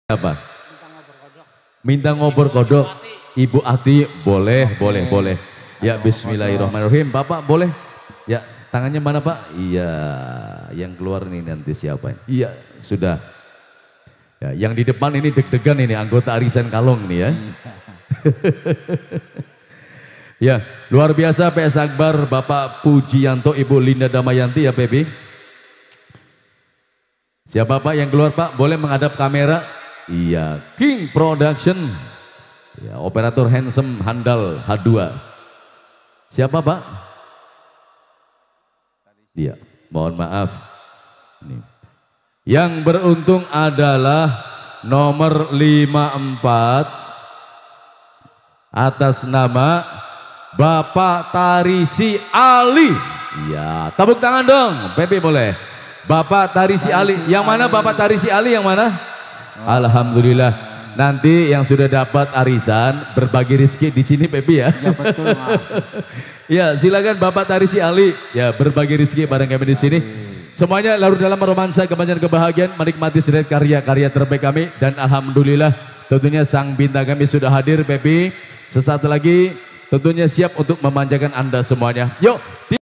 KECLEK ARISAN GROUP KALONG MARGADANA TEGAL JATENG 2022
HAJAT Bpk. Puji Yanto & Ibu Linda Damayanti
PELAKSANAAN
Kamis, 26 Mei 2022
TEMPAT
Kel. Cabawan Rt. 02/04 Kec. Margadama
Kota Tegal - Jawa Tengah